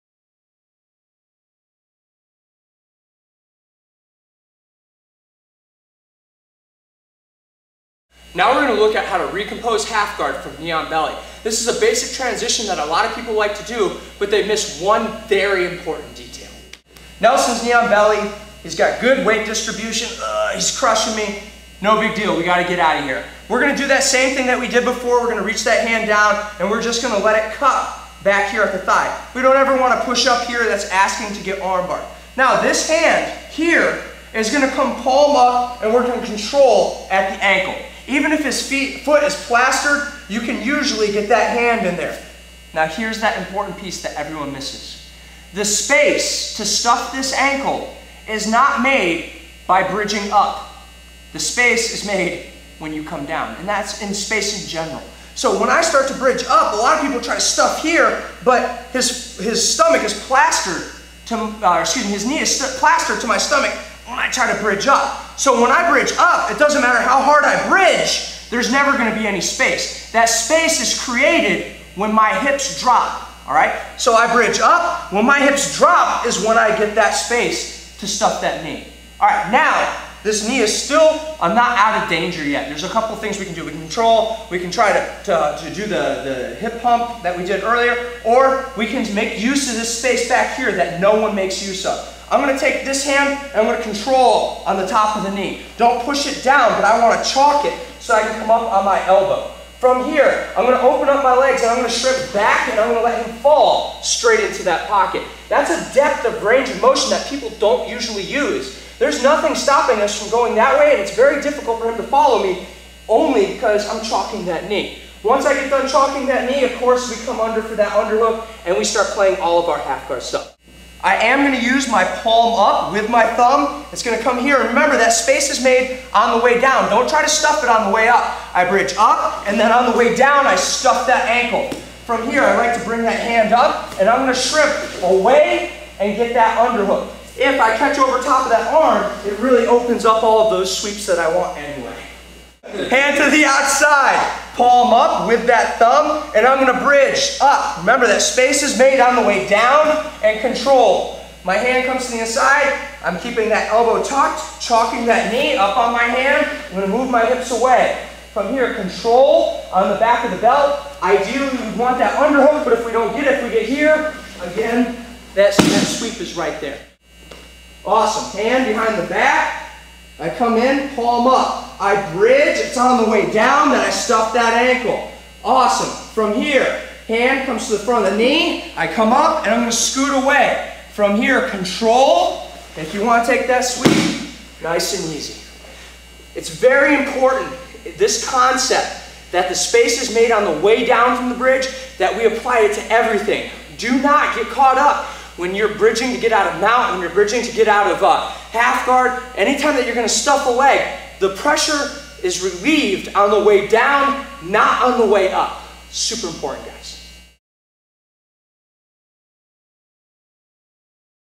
Knee on Belly Escape Stuff the Ankle
In this video I show another great way to escape the Knee on Belly position.

If you have ever been stuck under a skilled person that likes to play KOB then you understand the importance of escaping this position.  There are a bunch of really great techniques but this is probably the most commonly taught and easiest to do against a skilled opponent.

One of the very important concepts to remember is that the space is created on the way DOWN from the bridge, not the way up.  If you are having a hard time understanding what I'm saying it is a great idea to watch this video.

Upon watching this video you will also find another unique concept/technique.  When my opponent has his knee up in Combat Base or sitting on my thigh I like to shrimp my hips away to get their knee to the floor.  This is super easy and I rarely, if ever, see anyone do this.  Keep in mind you have a lot of space behind you that is not yet controlled by your opponent, make use of that space instead of battling over the space that you are both occupying.